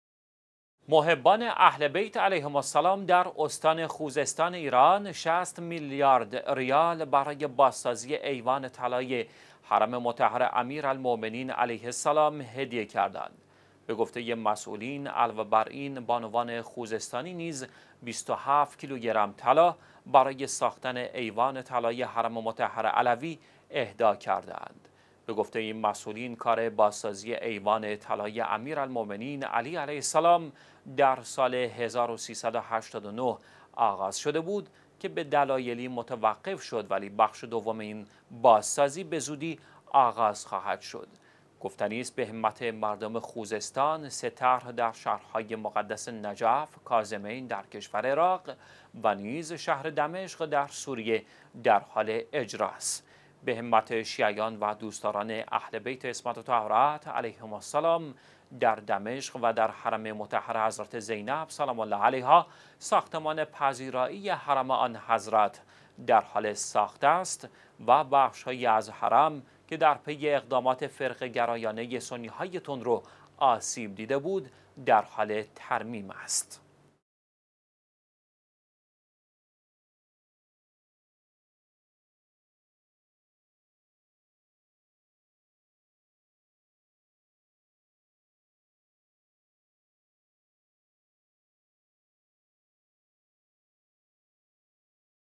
کمک ۶ ميليارد توماني خوزستاني‌ها براي بازسازي ايوان طلاي نجف
دوست داران «اهل_بیت علیهم السلام» در استان خوزستان ایران۶۰ میلیارد ریال برای بازسازی ایوان طلای حرم مطهر «امیرالمومنین علیه السلام» اهدا کردند.

به گفته مسئولین امر، علاوه بر این بانوان خوزستانی نیز ۲۷ کیلوگرم طلا برای ساخت ایوان طلای حرم مطهر علوی اهدا نموده اند.

به گفته این مسئولین، کار بازسازی ایوان طلای «امیرالمومنین علیه السلام» سال 1389 آغاز شده بود که به دلایلی متوقف شد، ولی بخش دوم این بازسازی به زودی آغاز خواهد شد.

گفتنی است که سه طرح در اماکن مقدسه به همت مردم خوزستان در شهرهای مقدس نجف، کاظمین در کشور عراق و شهر دمشق در سوریه در حال اجراست.

در دمشق و در حرم مطهر حضرت_زینب سلام الله علیها به همت شیعیان دوست داران «اهل بیت علیهم السلام» ساختمان پذیرایی آن حضرت در حال ساخت بوده و بخش هایی از حرم که در پی اقدات فرقه گرایانه سنی های تندرو آسیب دیده بود، در حال ترمیم است.